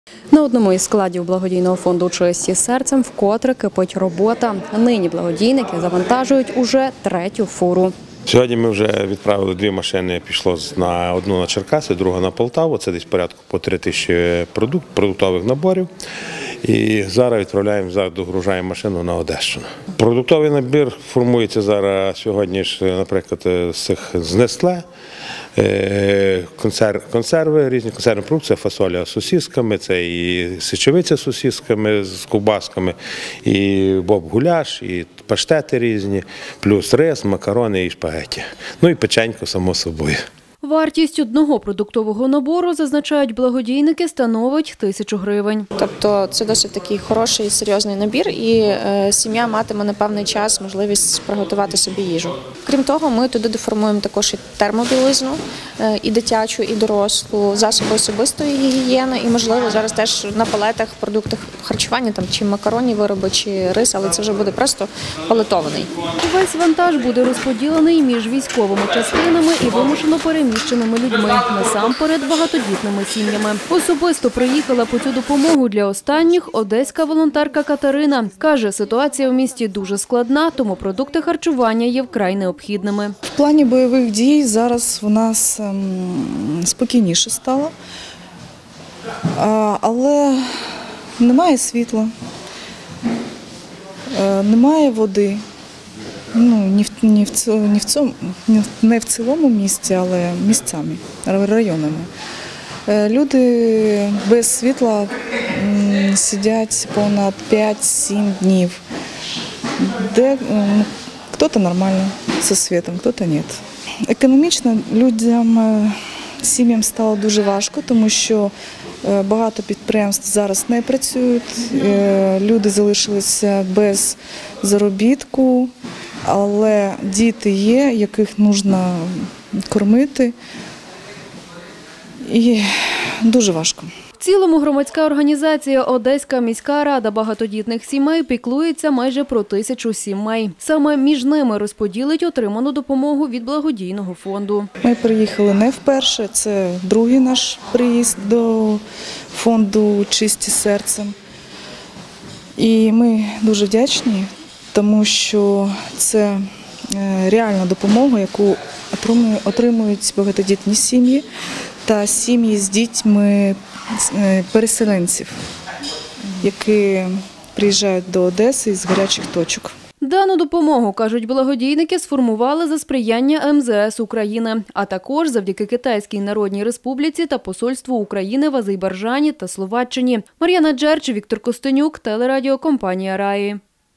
БФ "Чисті серцем" відправив чергову благодійну допомогу в Одеську, Полтавську, Черкаську області
Ввесь вантаж буде розподілений між військовими частинами і вимушено переміщеними людьми, насамперед багатодітними сім’ями.